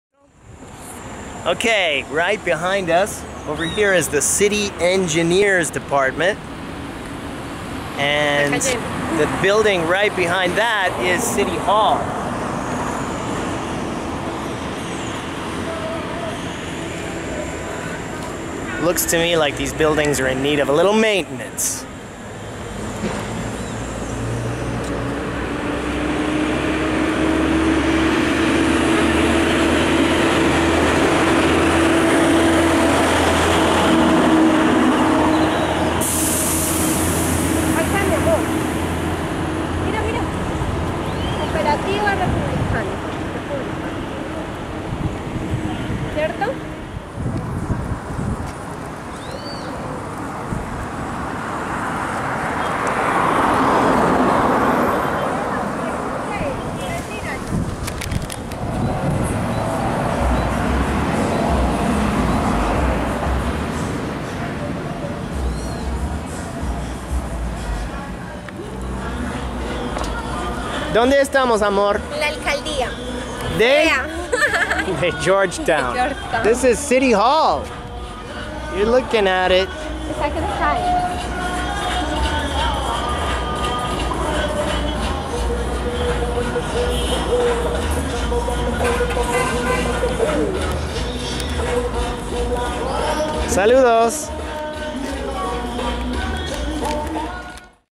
Georgetown City Hall, Guyana
Georgetown City Hall is a nineteenth-century Gothic Revival building located on the corner of Regent Street and Avenue of the Republic in Georgetown, Guyana. The building was designed by architect Reverend Ignatius Scoles in 1887, and was completed in June 1889. The building houses the offices of the Mayor, the City Council, and the City Engineer.